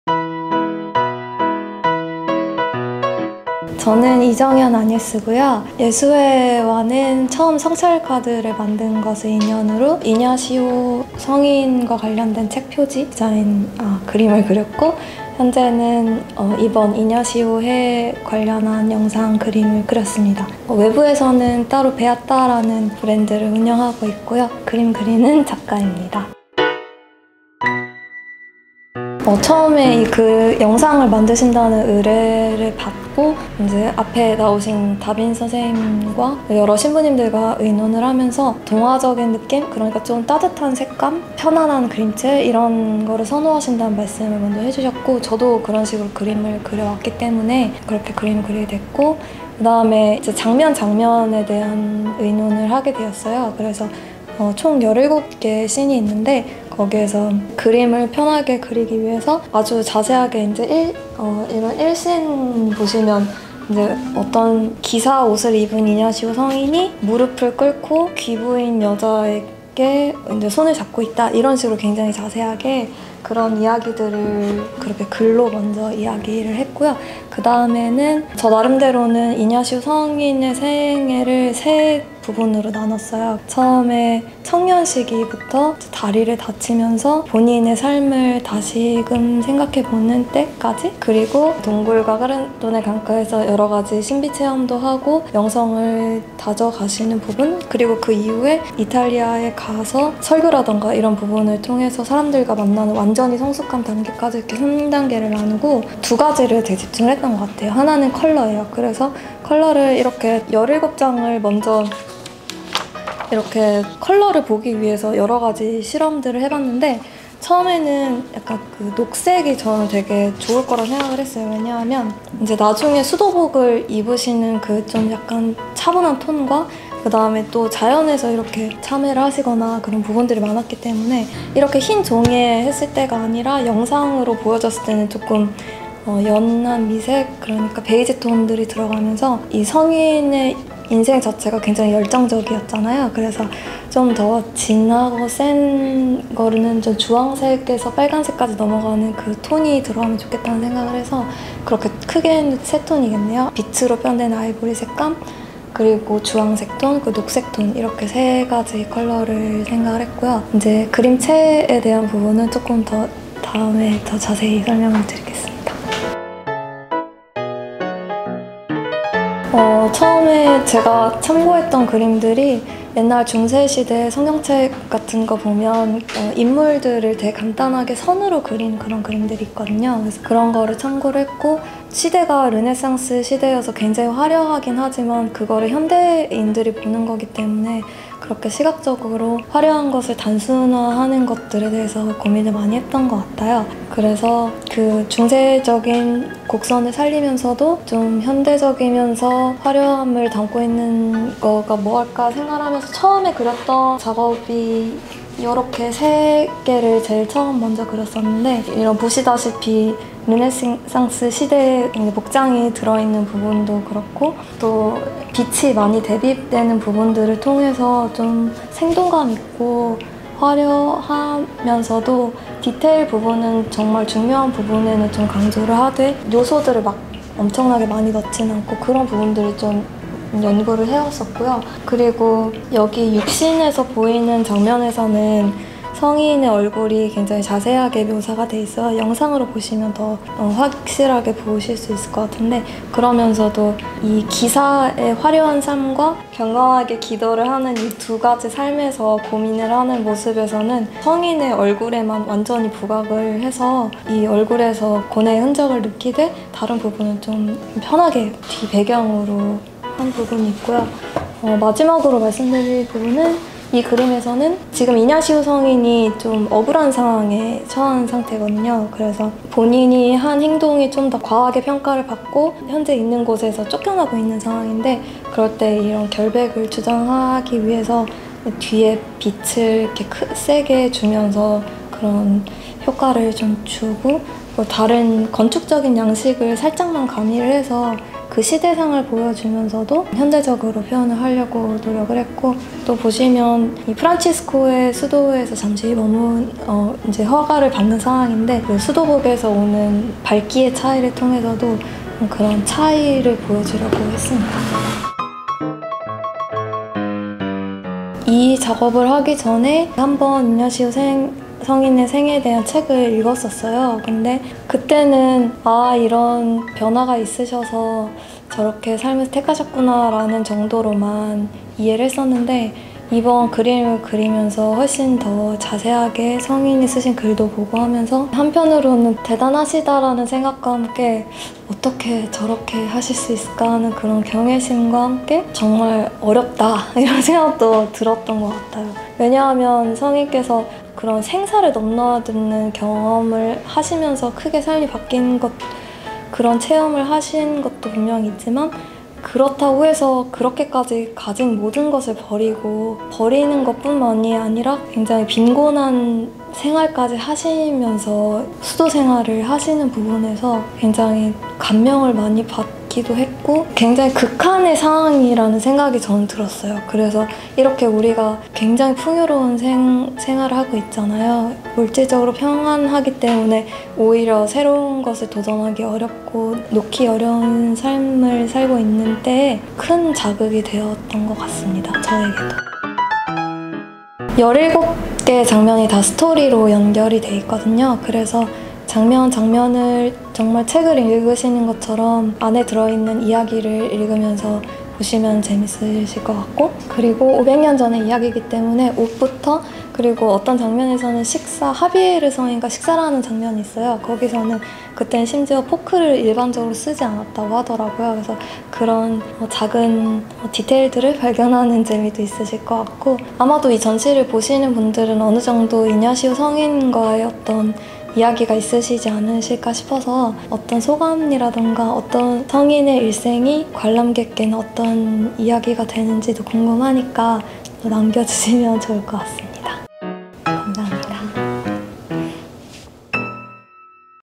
(ENG Sub.) [이냐시오의 해 두번째 기획전시] CAMINO IGNACIANO (이정현 아녜스 인터뷰)
이냐시오 회심 500주년을 맞아 [CAMINO IGNACIANO] 전시가 10월 15일(금)부터 마지스 예수회 청년 센터에서 열립니다~

이냐시오 성인이 걸어온 길을 작품을 통해 살펴보고
저희들의 삶도 돌아보는 은총 가득한 시간 되시길 바라요 *_*

📍장소: 마지스 예수회 청년 센터 (서울특별시 마포구 대흥동 서강대길 19)
🖼전시 기간: 10월 15일(금) ~ 12월 17일(금) (변동 가능*)
🎨전시 시간: 월~금 오후 1시~5시

작가: 이정현 아녜스
큐레이션: 레지나
작품 설치: 레지나, 흔관 SJ
촬영: 레지나, 베체
영상 편집: 베체
포스터: 리따